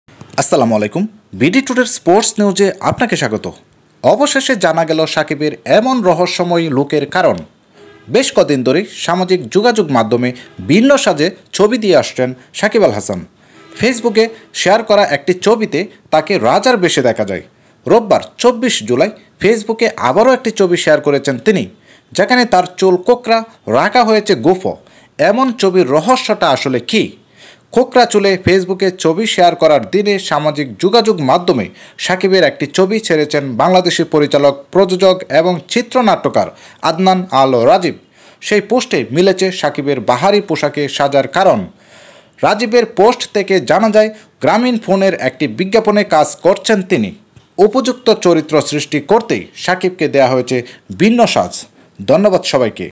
What's the reason Shakib al hasan new look?
Bd Today is a sports News Channel.

The reason for Shakib's look is known

Shakib Al Hasan has been giving pictures in 'different clothes' on social media for several days. In a picture shared on Facebook, he is seen dressed as a king. On Sunday (July 24) he shared a picture again on Facebook. Where his hair is curly, a mustache is also kept. What is the secret of such a picture?

Bangladeshi director, producer and screenwriter Adnan Al Rajeeb released a picture of Shakib on social media on the day of sharing the picture on Facebook with curly hair. In that post, the reason for Shakib's dressing in Bahari clothes is found. It is known from Rajeev's post that he is working on an advertisement of Grameenphone. Shakib has been given a different outfit to create a suitable character.